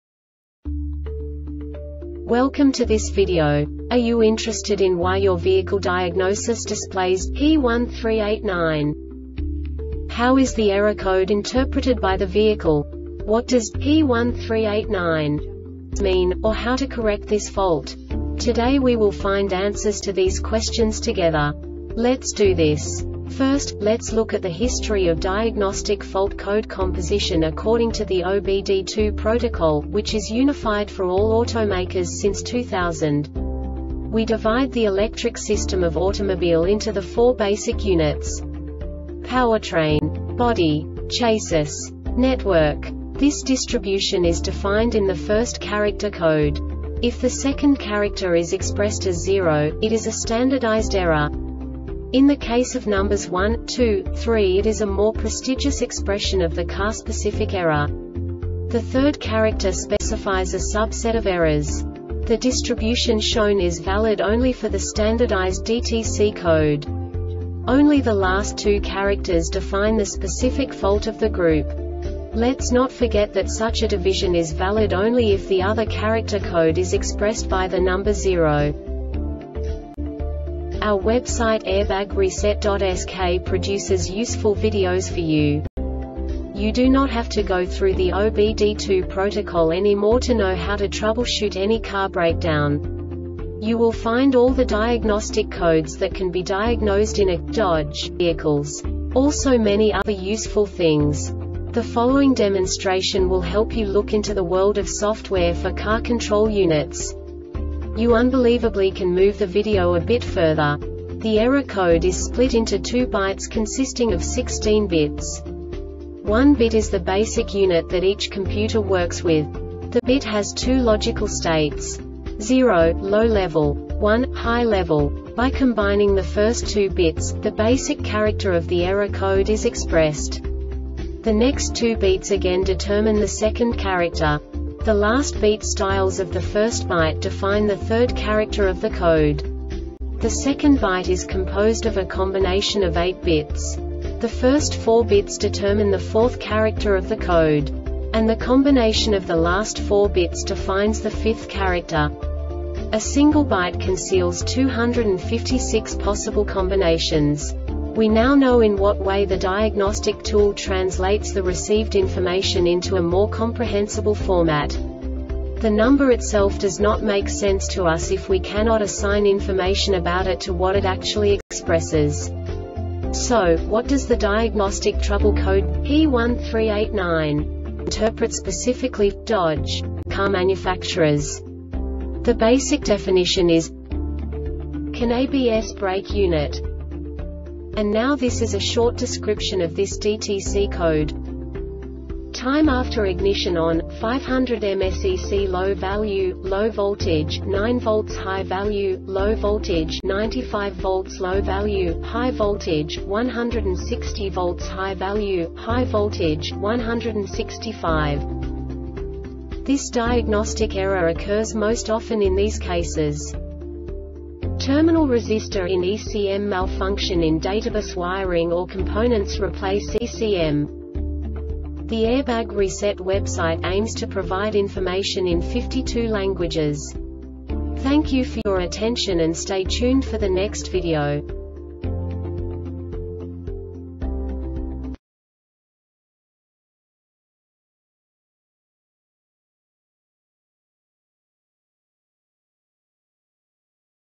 DTC Dodge P1389 Short Explanation
Contents:
0:21 Basic DTC analysis according to OBD2 protocol standard.
1:48 Insight into programming
2:58 A diagnostically understandable description of the Diagnostic Trouble Code
3:05 Basic error definition

Make:
Dodge
Code:
P1389
Definition:
CAN ABS brake unit
Description: